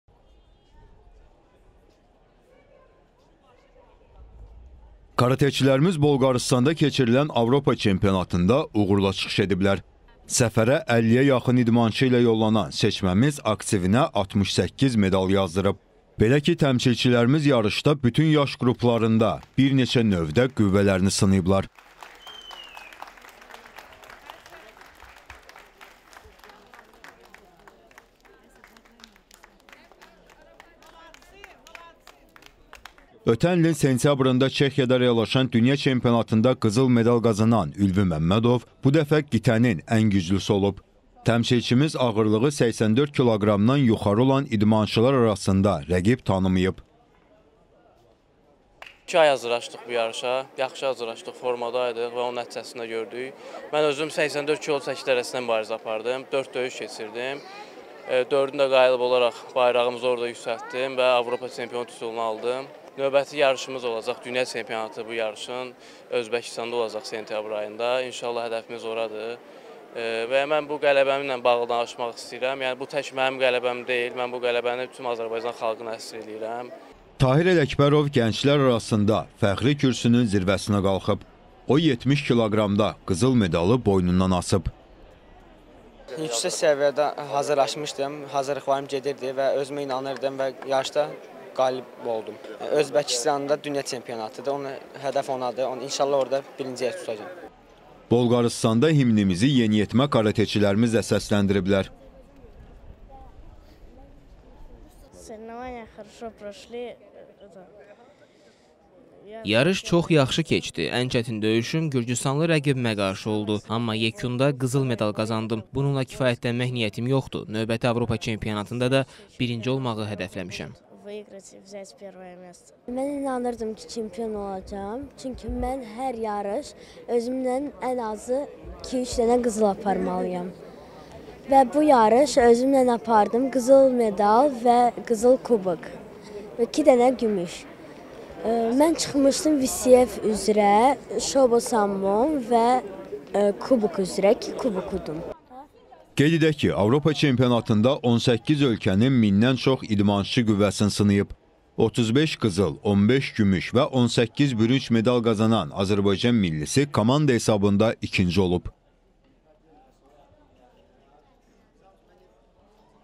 Karateçilərimizin Bolqarıstan zəfəri
Millimizin üzvləri Avropa Çempionatında uğurla çıxış ediblər